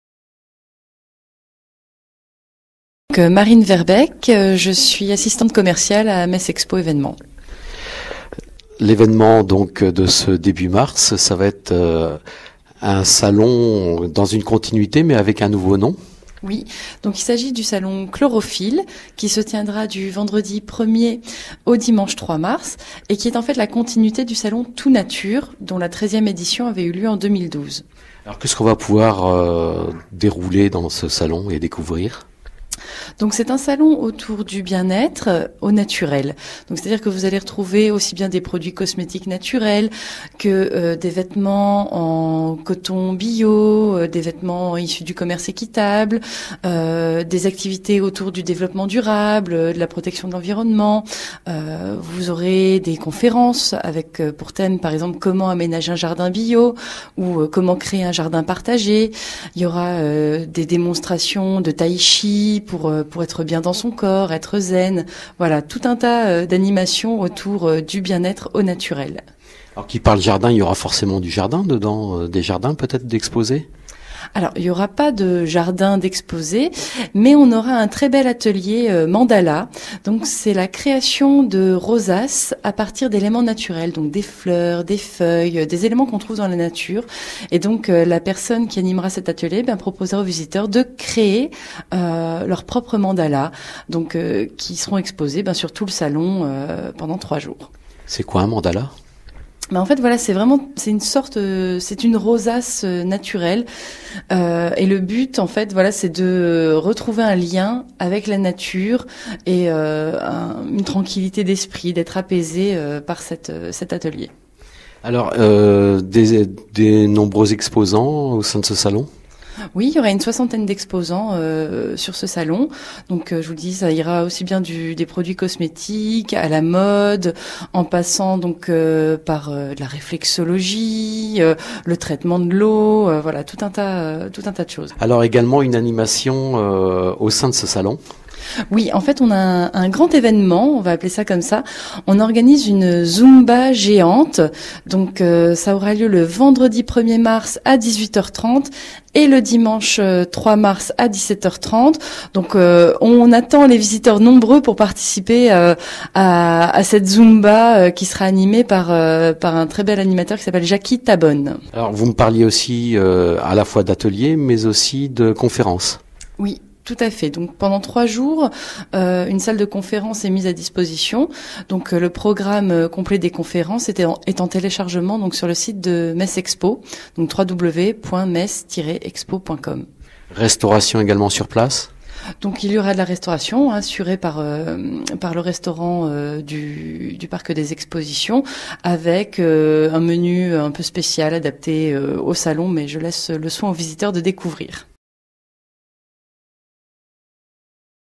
Metz Expo : Salon CHLORO'FEEL 2013
Salon CHLORO'FEEL
du vendredi 1er mars au dimanche 3 mars 2013
au Parc des Expositions de Metz Métropole
Chloro'Feel, la verte attitude à portée de main !
Chloro'Feel propose différentes manières d'adopter un mode de vie plus
respectueux de l'environnement, des idées pour consommer autrement et
prendre soin de soi au naturel.
Cet événement convivial est un lieu de rencontre et d'échange pour toute la
famille. Les professionnels et associations présents viennent à la rencontre du
public pour présenter leur savoir-faire, leurs produits et services, échanger et
prodiguer leurs conseils.
Différents secteurs seront représentés : marché Bio (produits alimentaires, boissons
et vins bio), beauté et cosmétiques, bien-être / forme / santé, mode bio et éthique,
habitat naturel, jardinage, artisanat et commerce équitable, édition / presse / musique,
tourisme vert et solidaire / loisirs
Des conférences
· Les bienfaits d'une alimentation végétale
· Commencer et aménager un jardin Bio
· Créer un jardin partagé
· etc
Des ateliers :
· A la découverte des plantes sauvages comestibles
· Démonstration de Fit'Poussette : cours de remise en en forme avec bébé
· Mandala éphémère
· Taï-chi-chuan
Programmes complets des conférences et ateliers disponibles rubrique
Chloro'Feel
Sans oublier la ZUMBA® PARTY qui se tiendra le vendredi 1er mars à 18h30 et le
dimanche 3 mars à 17h30. Jacky Tabone, professeur de danse et instructeur de
Zumba®, emmènera les visiteurs vers la joie et la santé lors d'un cours exceptionnel.
Un jeu tirage au sort sera également organisé sur le salon. De nombreux lots à
gagner : des entrées au Parc Animalier de Sainte Croix et aux Jardins Fruitiers de
Laquenexy, des coffrets cadeau Lush, des livres offerts par les Editions Fleurus, des
Informations pratiques :
Horaires : Vendredi 11h--20h, samedi 10h-20h et dimanche 10h-19h.
Metz Expo Événements- Parc des Expositions de Metz Métropole -- Rue de la Grange aux Bois -- BP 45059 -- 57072 Metz Cedex 03